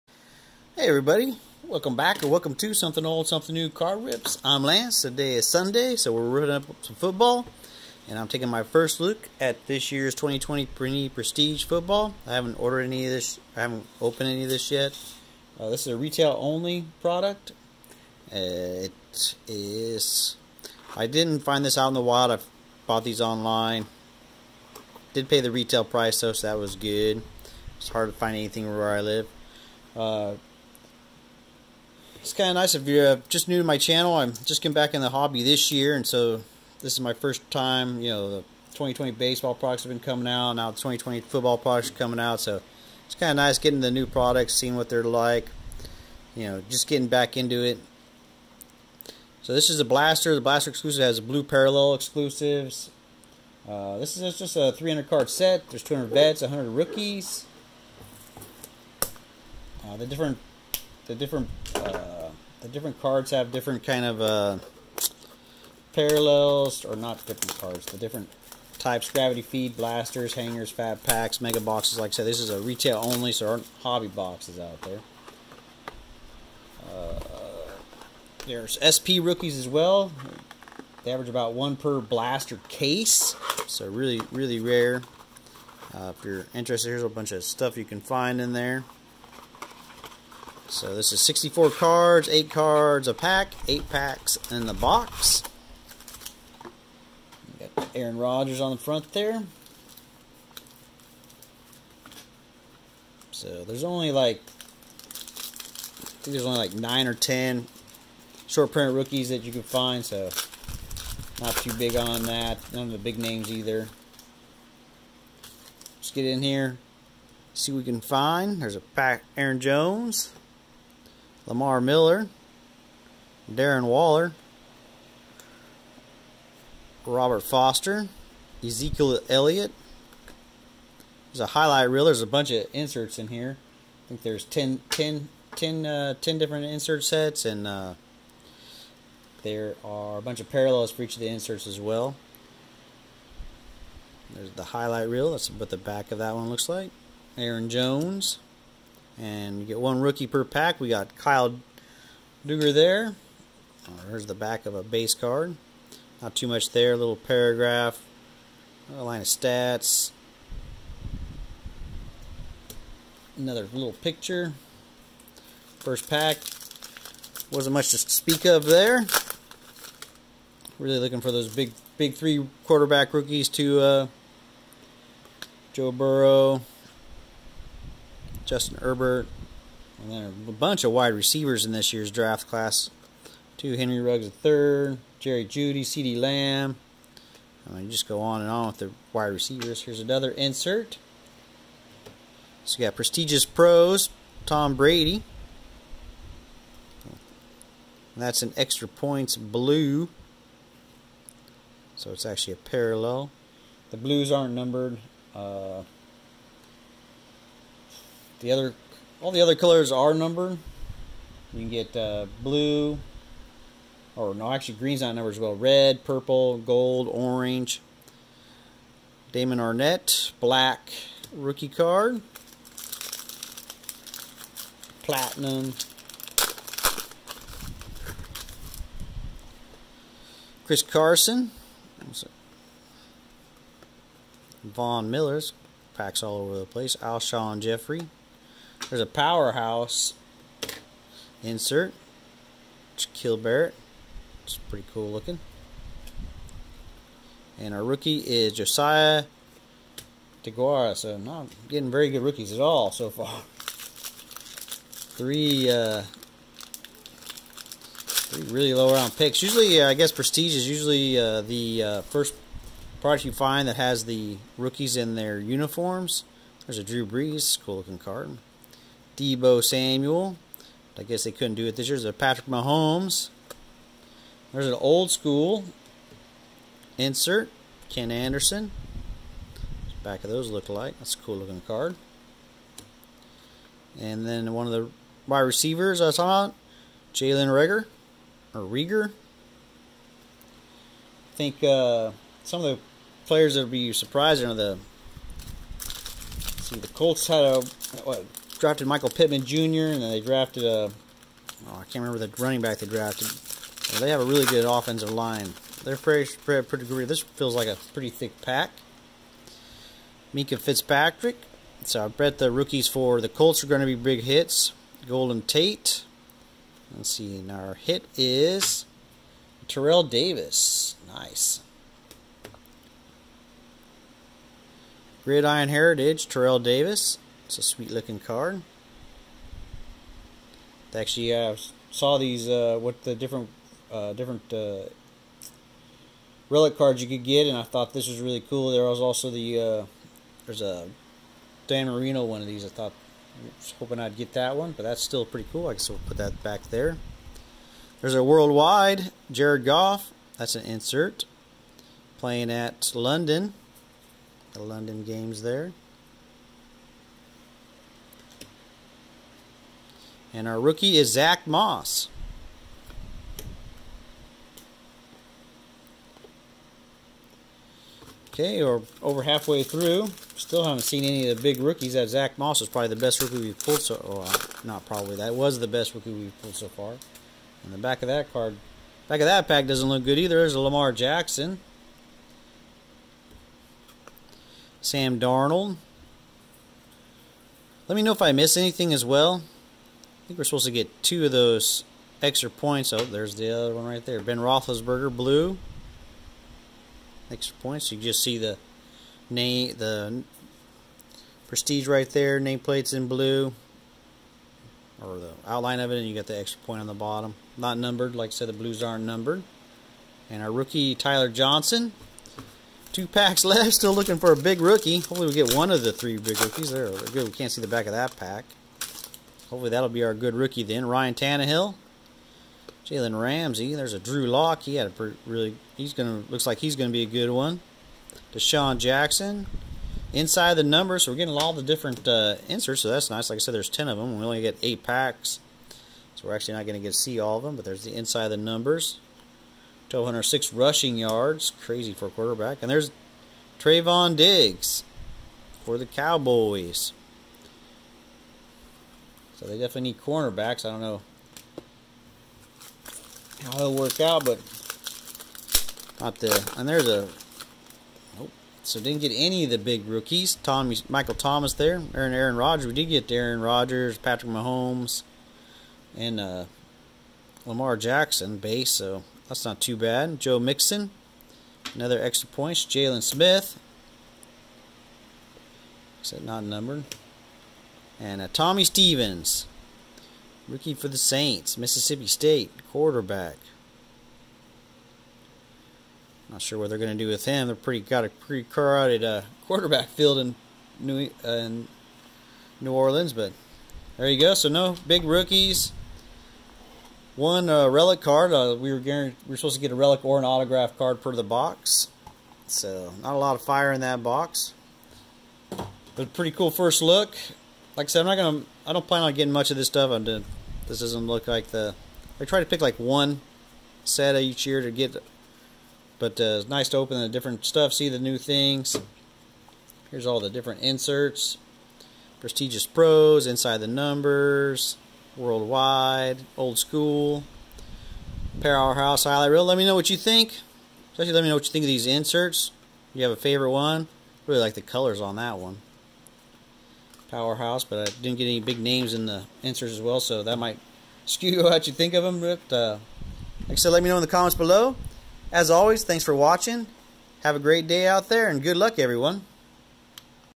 2020 Panini Prestige Football - 1st look (last look?)
Hey everyone, on this fine summer Sunday we are ripping into a newer football card product. We have a blaster box of 2020 Panini Prestige. This is a retail exclusive product. We are looking for all of the big rookies, a lot of inserts (10 sets) and hoping to get a nice auto out of here. Short story even shorter, this box was kinda a snooze fest and even the inserts couldn't save it. Check it out and let me know what you think.

What insert set out of this product do you like the best?